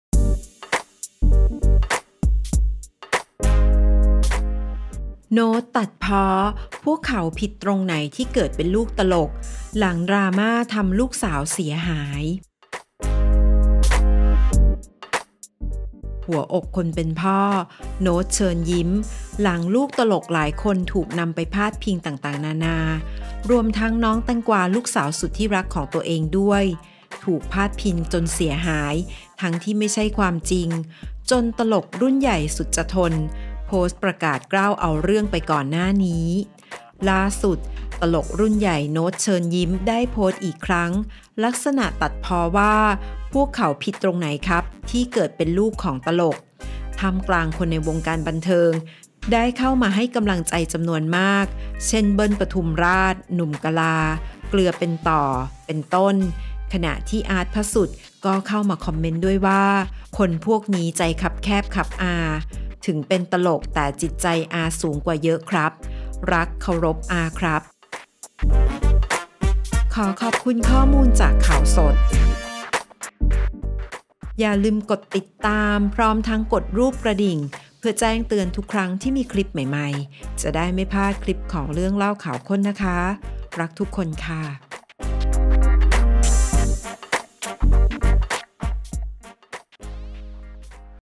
โน้ต เชิญยิ้ม ตัดพ้อ! พวกเขาผิดตรงไหน ที่เกิดเป็นลูกตลก หลังดราม่าทำลูกสาวเสียหาย
หัวอกคนเป็นพ่อ โน้ต เชิญยิ้ม หลัง ลูกตลกหลายคน ถูกนำไปพาดพิงต่างๆ นานา รวมทั้ง น้องแตงกวา ลูกสาว สุดรัก ของตัวเองด้วย ถูกพาดพิง จนเสียหาย ทั้งที่ไม่ใช่ความจริง จน ตลกรุ่นใหญ่ สุดจะทน โพสต์ประกาศกร้าวเอาเรื่อง ไปก่อนหน้านี้